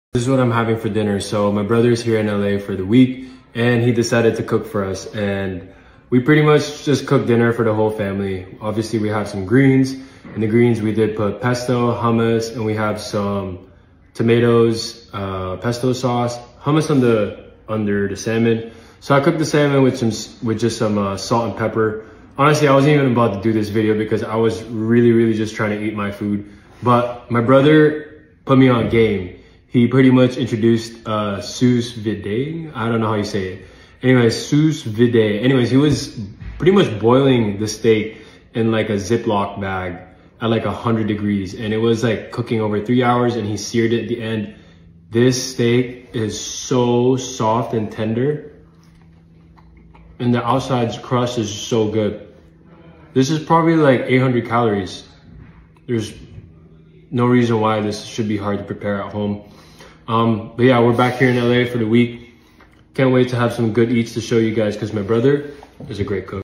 It’s been a month of good eats, promise I’ll be doing more art again
It’s been a month of good eats, promise I’ll be doing more art again. Kinda just been enjoying quality time with the fam, hope you guys are too ❤️